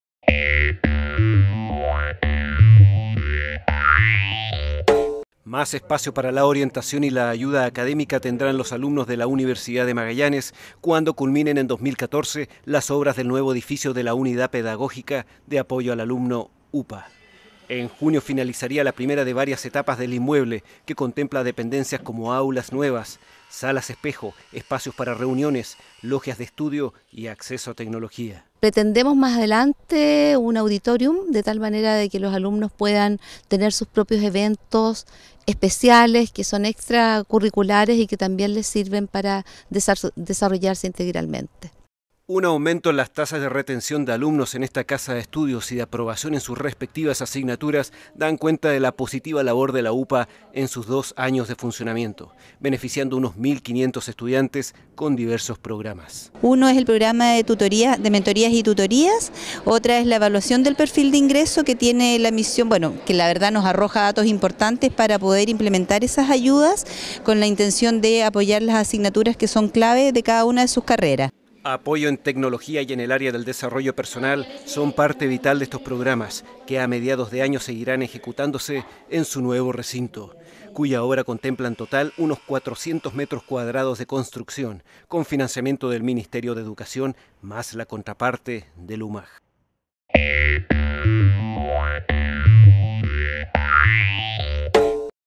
Nuevo edificio Unidad Pedagógica de Apoyo al Alumno UMAG
Más espacio para la orientación y la ayuda académica tendrán los alumnos de la Universidad de Magallanes, cuando culminen en 2014 las obras del nuevo edificio de la Unidad Pedagógica de Apoyo al Alumno, UPAA.En junio finalizaría la primera de varias etapas del inmueble, que contempla dependencias como aulas, una sala espejo, espacios para reuniones, logias de estudio y acceso a tecnología.